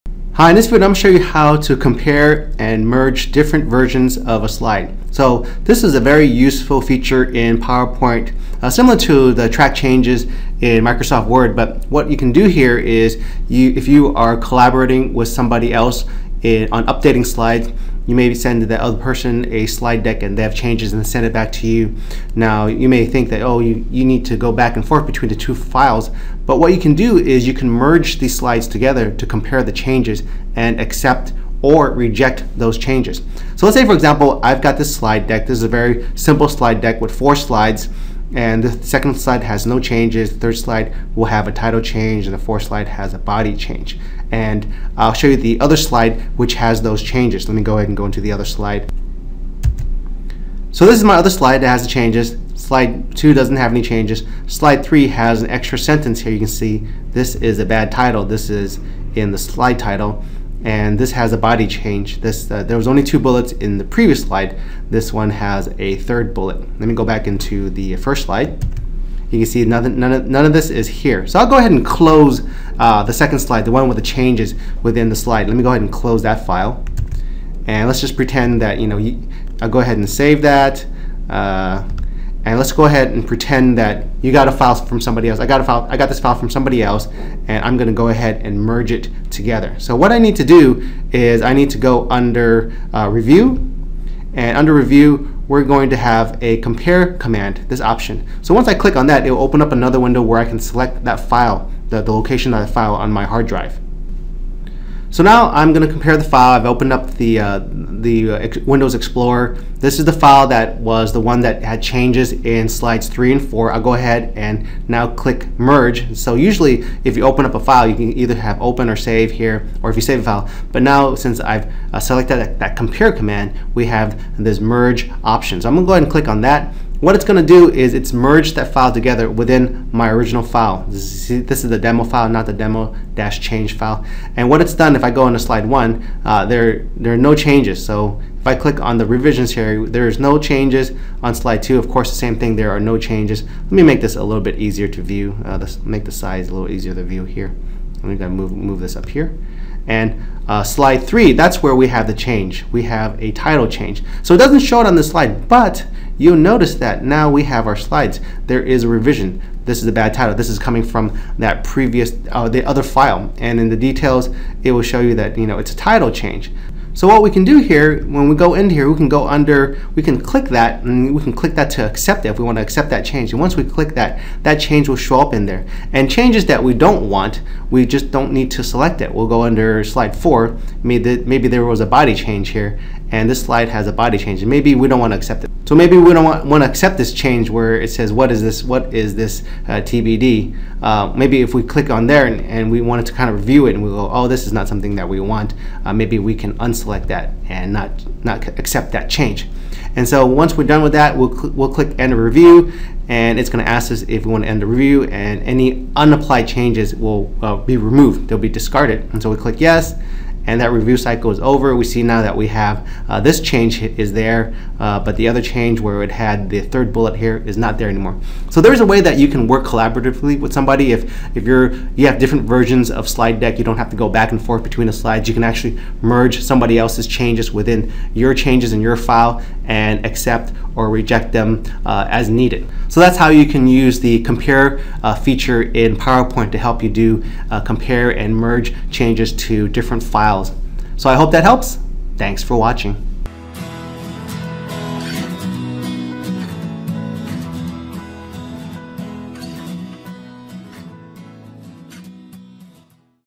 Compare and Review Changes Between Two Presentations
This video show how to compare changes between two different PowerPoint files.  This is a useful feature when you are working with someone else to collaborate on a presentation.  You can merge changes from another PPT into your file, review and accept/reject those changes.

P.S. Feel free to provide a comment or share it with a friend!